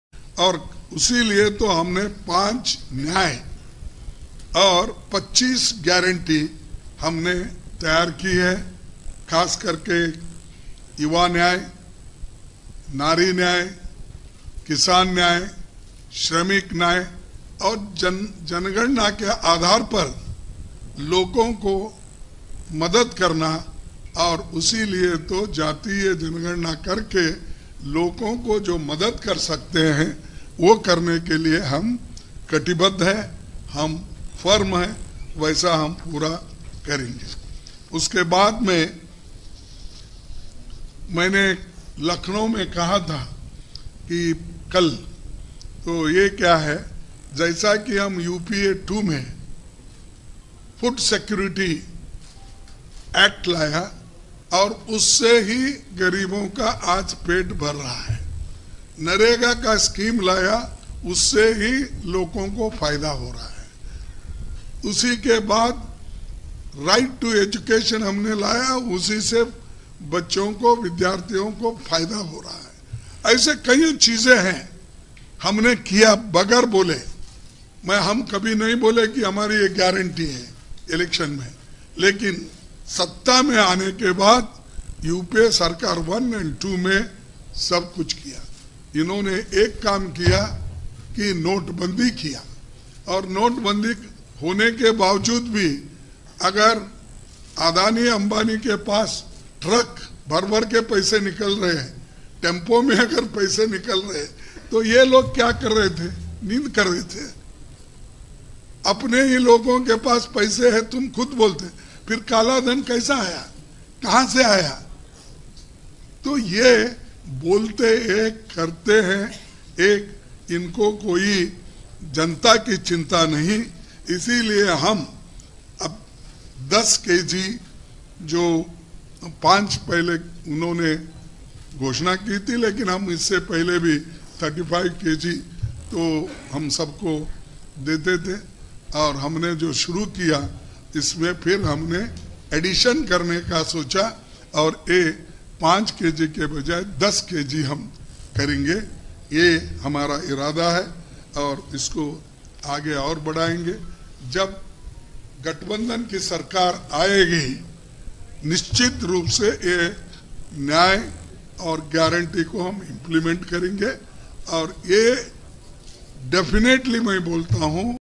कांग्रेस ने 5 न्याय और 25 गारंटी दी है।हमने युवा न्याय, नारी न्याय, किसान न्याय, श्रमिक न्याय और हिस
कांग्रेस ने 5 न्याय और 25 गारंटी दी है।

हमने युवा न्याय, नारी न्याय, किसान न्याय, श्रमिक न्याय और हिस्सेदारी न्याय की बात की है। हम जातिगत जनगणना भी जरूर करवाएंगे।

कांग्रेस फूड सिक्योरिटी स्कीम लाई, शिक्षा का अधिकार दिया और मनरेगा शुरू किया। हमने इसकी गारंटी नहीं दी, लेकिन ये सब कुछ किया।

हमारी सरकार आने पर हम 10 किलो राशन देंगे और सारी गारंटियों को पूरा करेंगे।

: कांग्रेस अध्यक्ष श्री kharge

📍 भुवनेश्वर, ओडिशा

Congress News
Rahul Gandhi News
Assam News